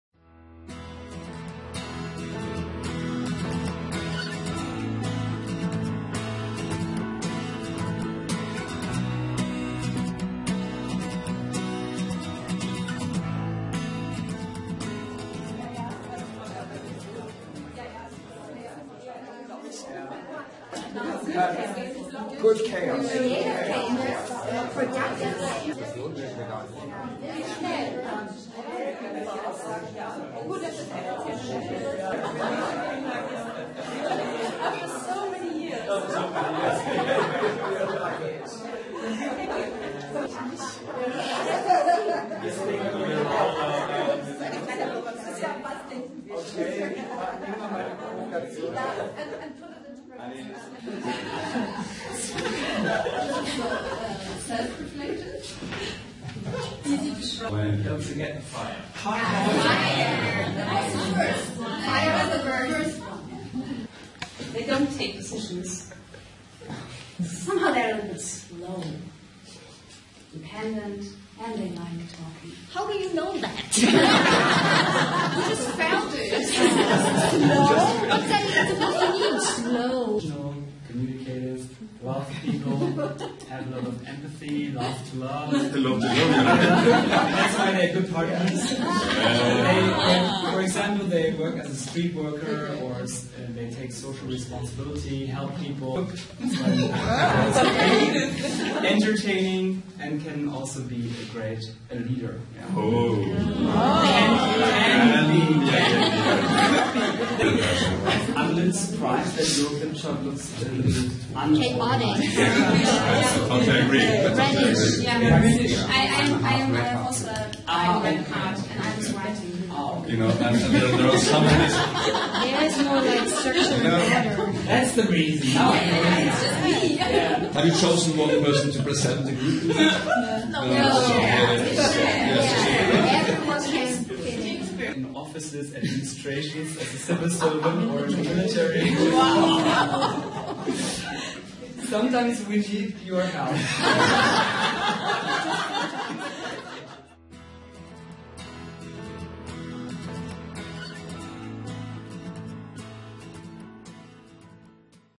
Diversity Icebreaker workshop
Diversity Icebreaker workshop video clips.
The Diversity Icebreaker workshops start with a questionnaire and quickly move on to engaging group activities. Enjoy!